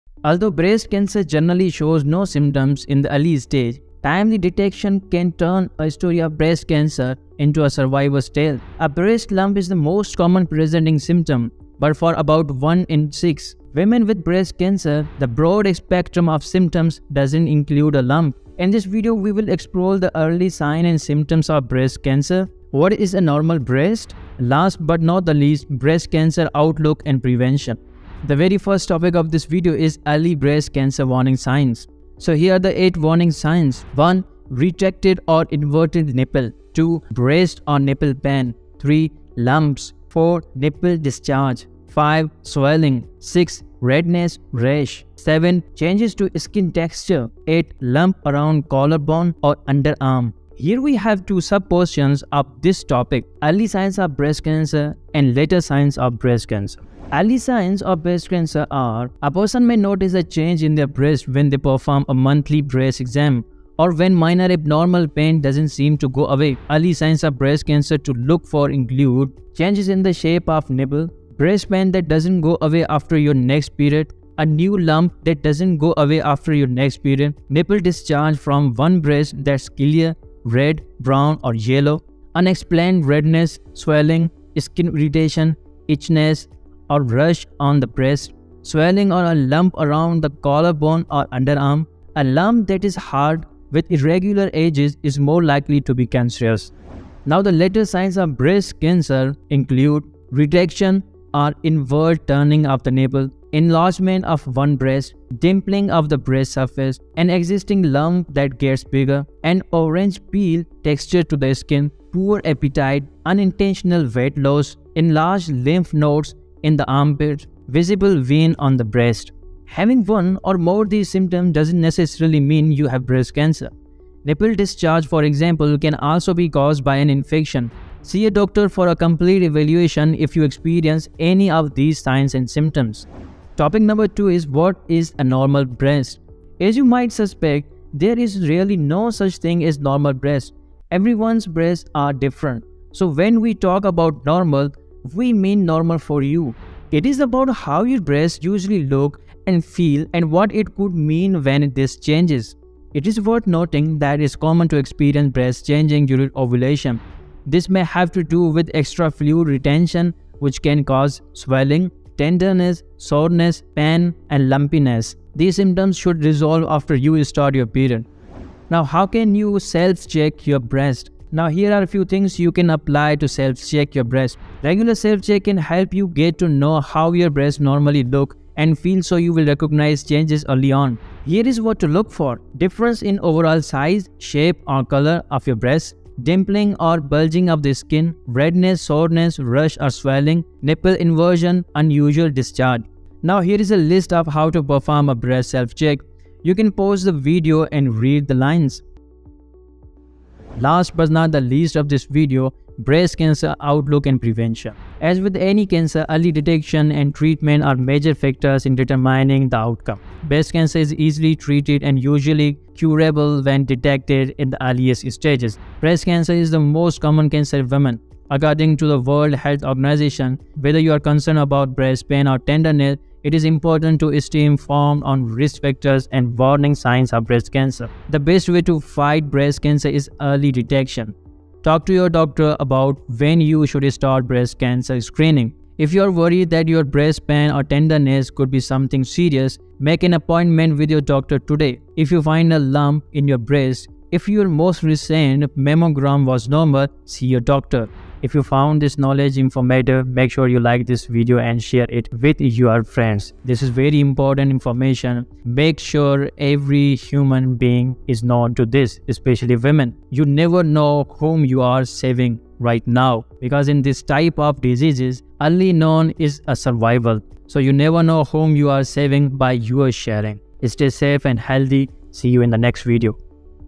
Sign & Symptoms of Breast Cancer | स्तन कैंसर के लक्षण | Every Girl Should Know!
Although breast cancer generally shows no symptoms in the early stage, timely detection can turn a story of breast cancer into a survivor’s tale.

A breast lump is the most common presenting symptom. But for about 1 in 6Trusted Source women with breast cancer, the broad spectrum of symptoms doesn’t include a lump.